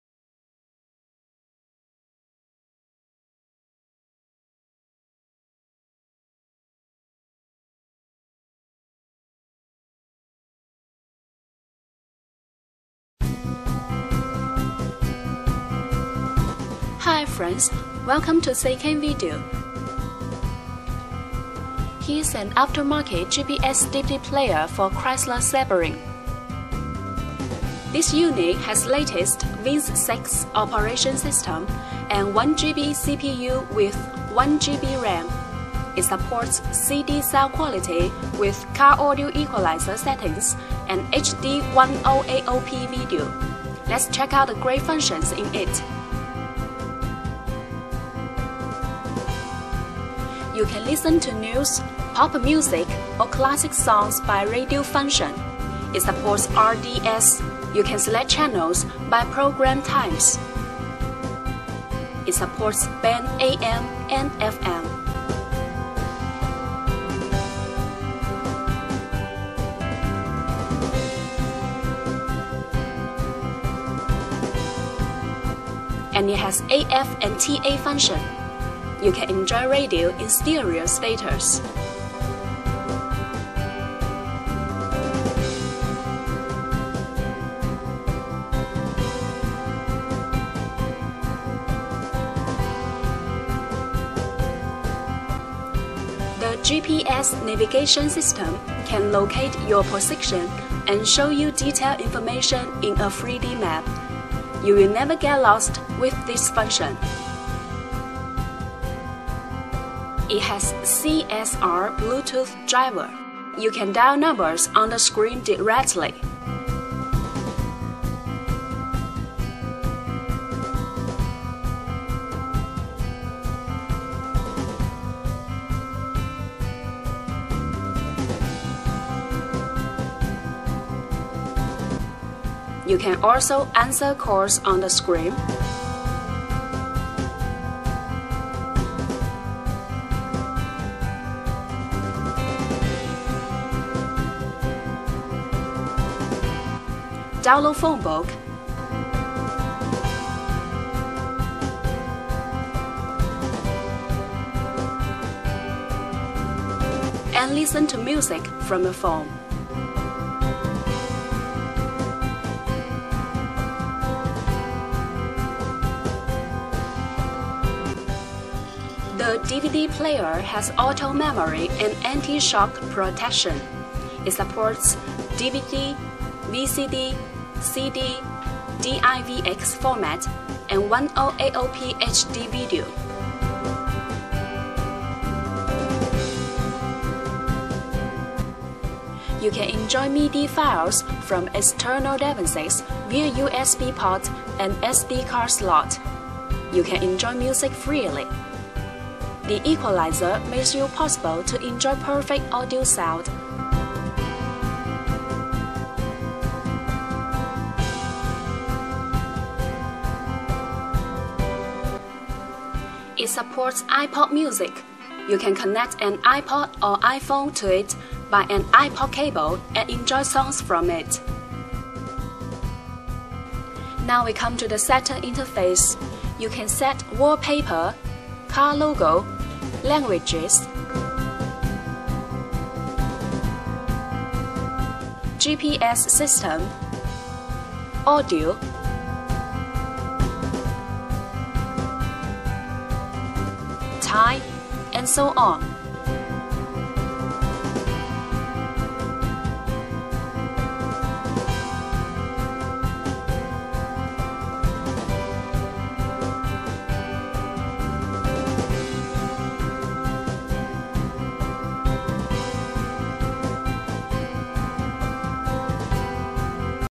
2002 2003-2006 Chrysler Sebring Aftermarket Radio TV Audio System Support 3D Map USB BT MP4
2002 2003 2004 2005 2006 Chrysler Sebring aftermarket radio TV audio system support 3D map USB BT MP4.
This unit has Cortex A9 Star 785 1GHz CPU with 1G RAM and 7 inch 800*480 2‐DIN WVGA HD Digital TFT LCD touch screen.
Functions: GPS: It supports 3D navigation map with cursors and voice cue.
Radio: It supports radio RDS/TA/AF/ST/LOC and 30 stations preset for AM/FM.
DVD: It has auto memory and supports DVD±R, DVD±RW, CD-R/RW, VCD, HDVD, DVCD, DIVX etc.
Bluetooth: It supports Bluetooth hand free, music, phone book and so on.
IPod: It has IPod function and also supports music from iPod Class, iPod Nano, iPhone4, iphone4s and so on.
AUX: It has Video/Audio/Aux input and output ports. You can connect to amplifier, headrest LCD monitors, rearview camera and other external media devices.